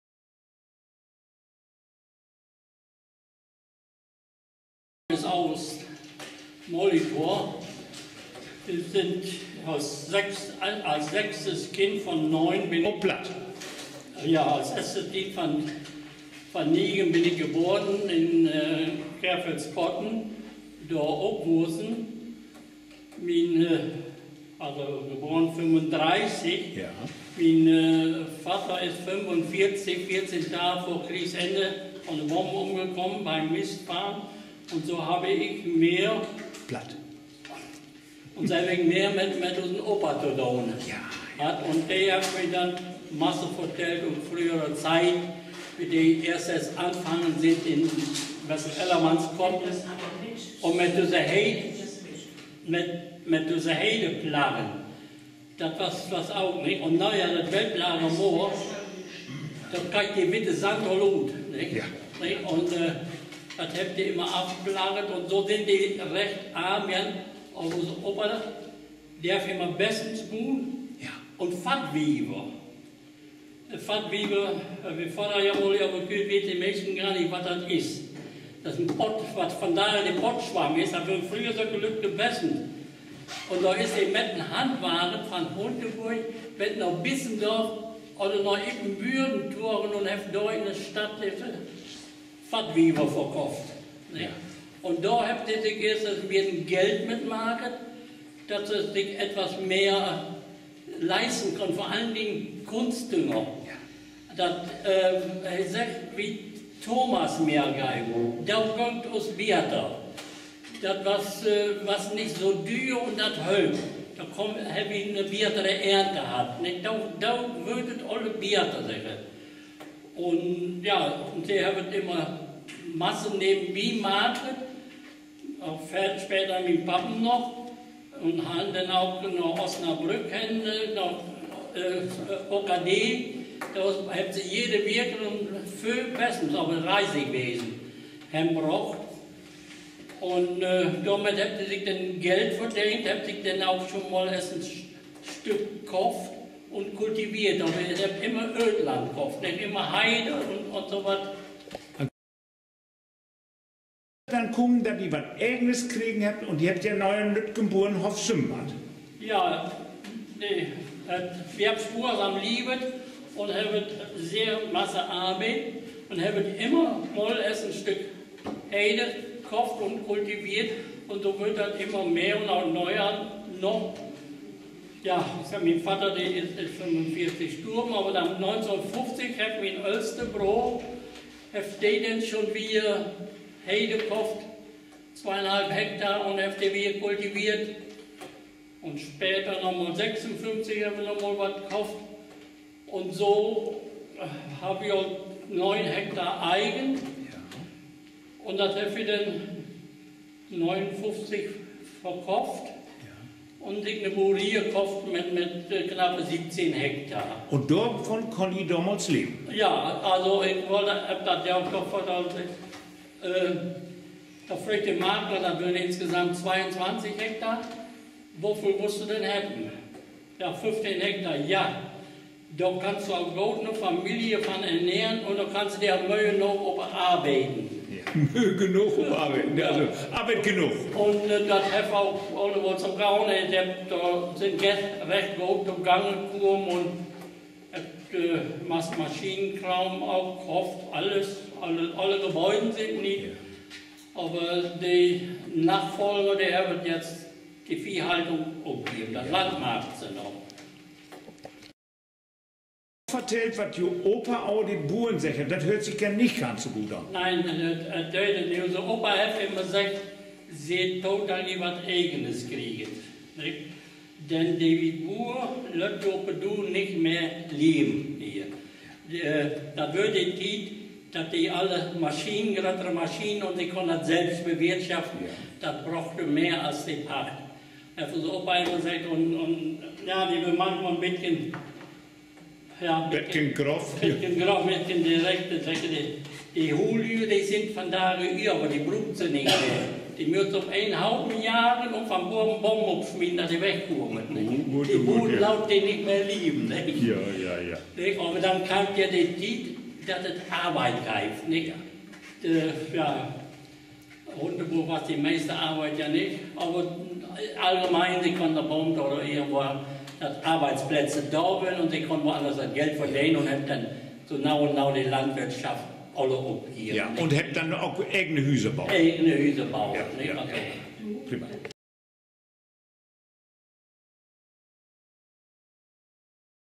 August Molitor aus Hunteburg zum Heuerlingswesen
August Molitor aus Hunteburg hat das Heuerlingswesen noch erlebt und er erzählt über diese schwierige Zeit.